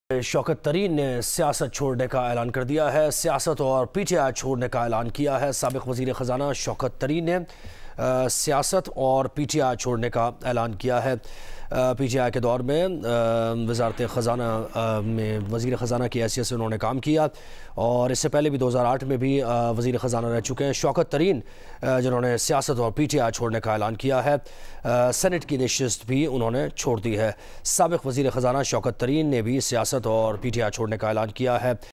Shaukat Tarin announcement to quit politics and PTI
Former finance minister ShaukatTarin announcement to quit politics and PTI | Geo News
سابق وزیر خزانہ کا سیاست اور پی ٹی آئی چھوڑنے کا اعلان